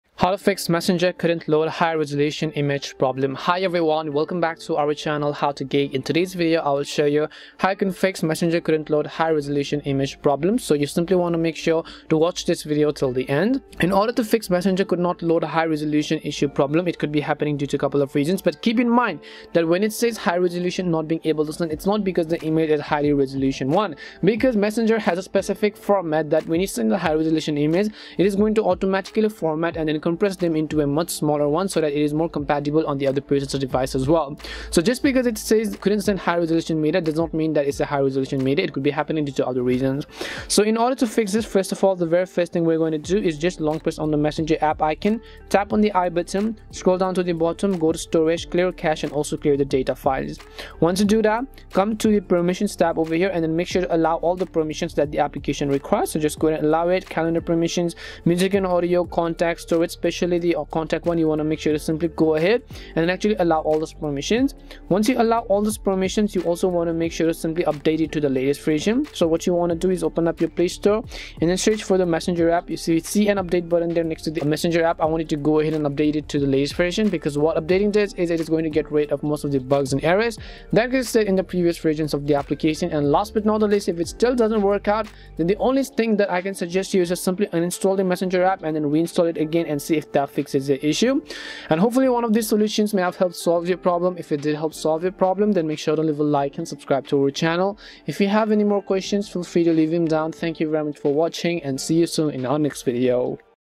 Fix Messenger Couldn't Load High Resolution Image Problem Solved
In this tutorial video, I will quickly guide you on how you can fix Messenger couldn't load high-resolution image problem. So make sure to watch this video till the end. We try to be responsive to as many questions as possible.

►  Chapters:
0:00 Introduction
0:13 It's Not What You Think!
0:40 General Fixes
1:31 Conclusion